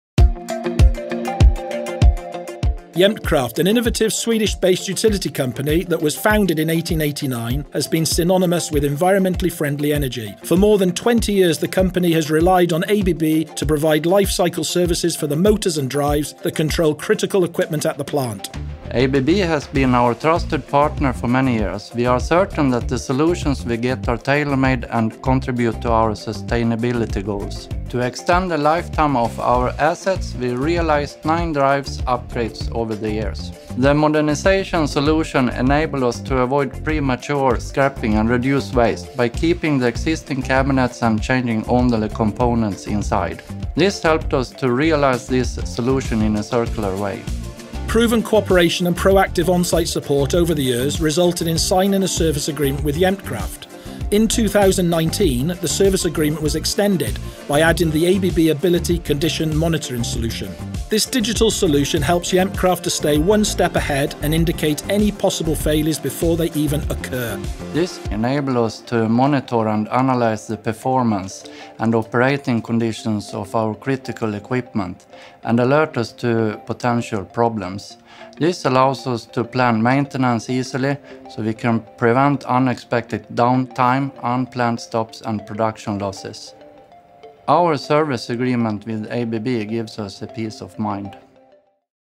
A trustful partnership with ABB helps Jämtkraft to extend assets’ lifetime and enable circularity
ABB provided life cycle services for the motors and drives that control critical equipment at Jämtkraft’s factory for more than 20 years. Performed modernization services prolonged the equipment’s lifetime and helped to avoid premature scrapping and reduce waste, by reusing the existing infrastructure. Proven long-term cooperation resulted in the signing of a service agreement between Jämkraft and ABB.